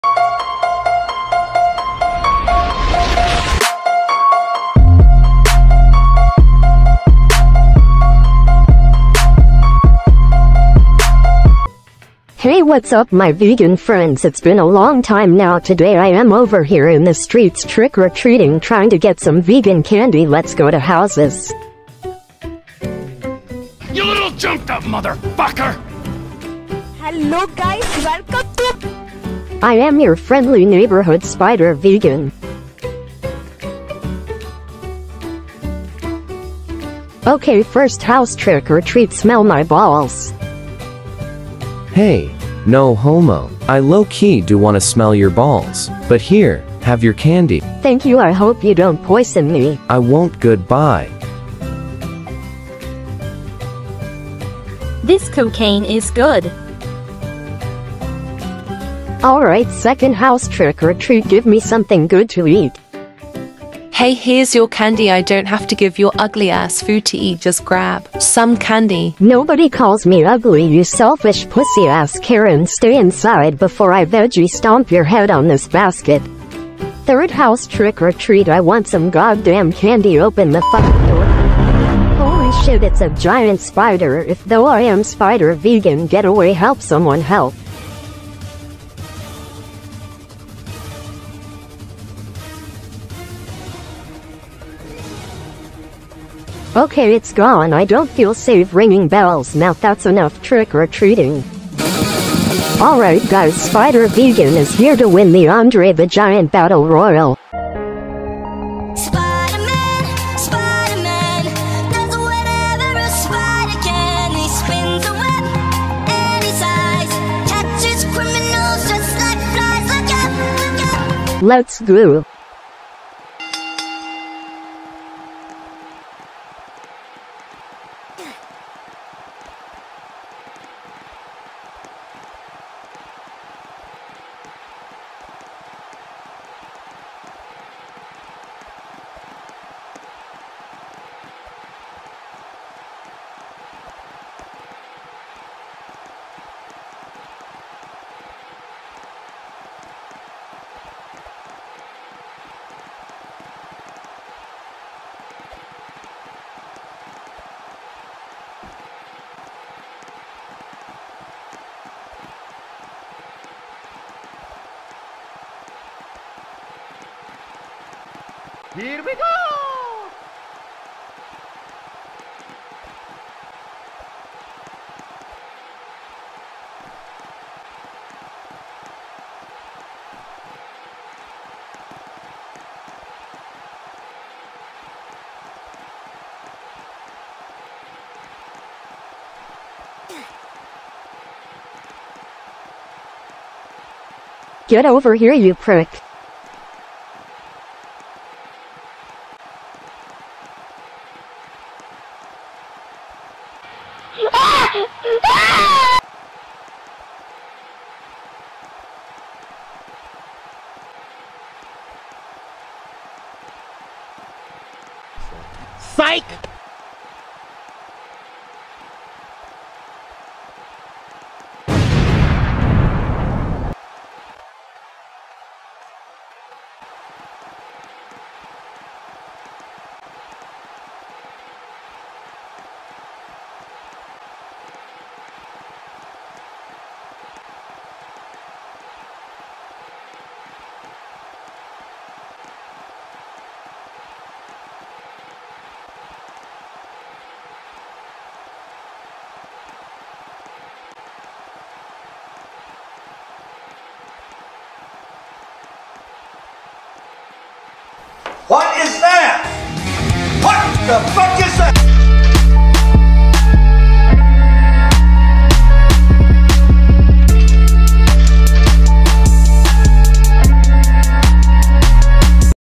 Can Vegan Rollins Win The Andre the Giant Battle Royale on Wrestlemania?
Your friendly neighborhood Spider Vegan goes trick or treating and tries to win The Battle Royal but can he do it?!